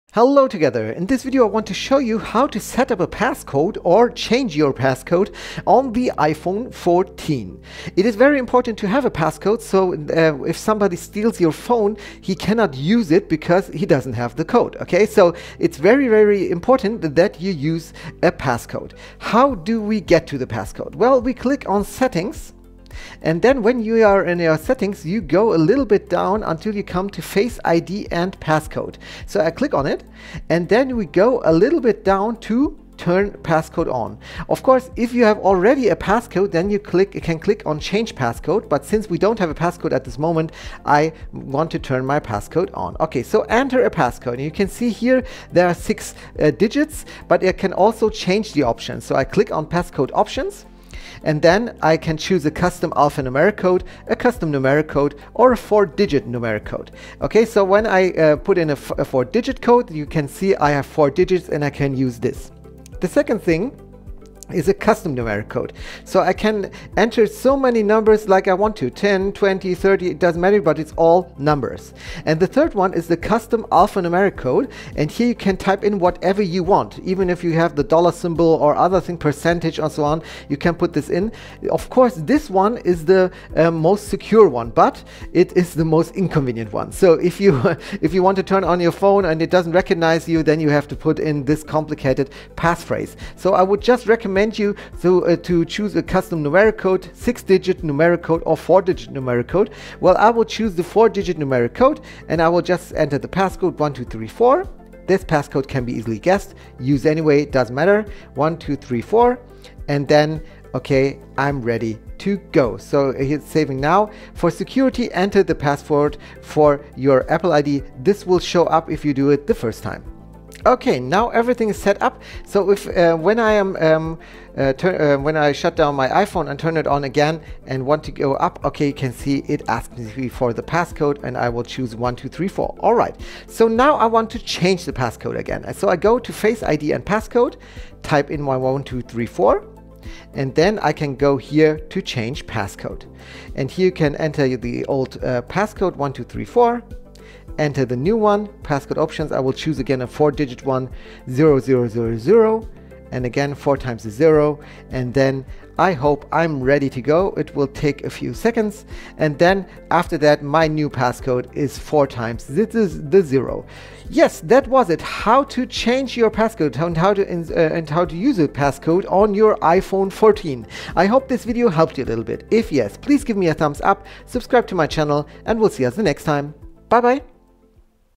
iPhone 14 - How to activate a passcode •  • 📱1️⃣4️⃣ • 🔢 • 🔐 • Tutorial
You have the iPhone 14 and want to protect it with a code or change the code you have been given? It's definitely a good decision to protect your device and your data with a code. Most of you have probably already done this in the initial setup of the iPhone 14, but perhaps you have skipped this step or simply want to change your code. Especially if you have the feeling that someone might have seen your code, it makes sense to change it. In this video, we show you how to protect your iPhone 14 with a code. Have fun with it.

FunFact:
With a four-digit code, there are a total of 10,000 different possibilities when using the numbers 0 - 9 - and you only ever have three attempts at a time 😉